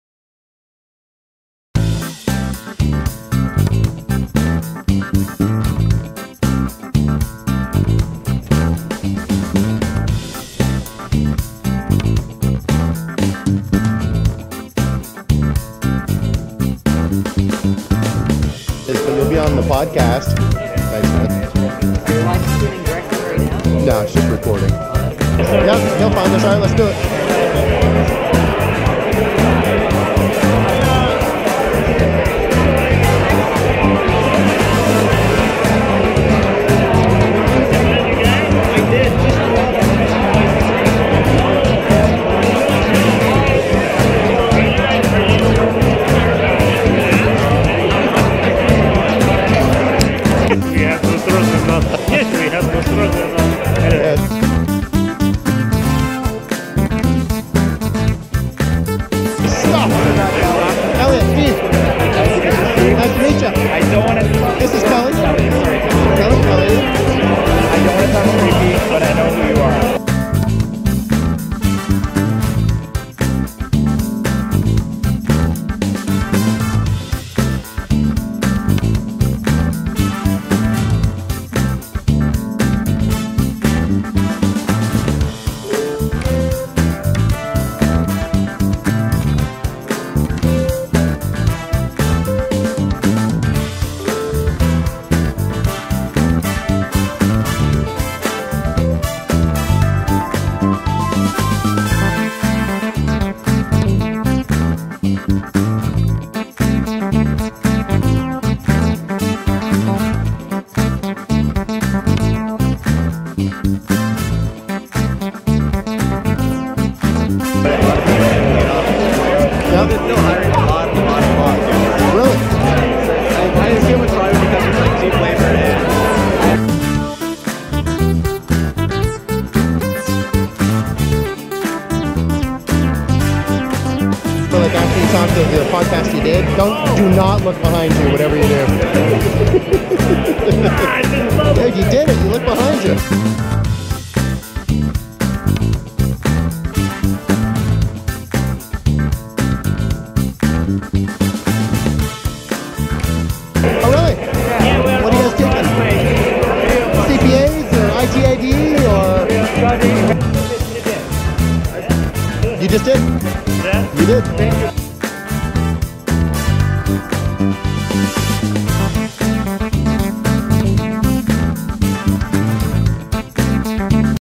HackerNest KW in action. Nerd Party Alert!! --Long Version--
@sirs_campbell attends the September HackerNest event in Kitchener/Waterloo, Ontario Canada.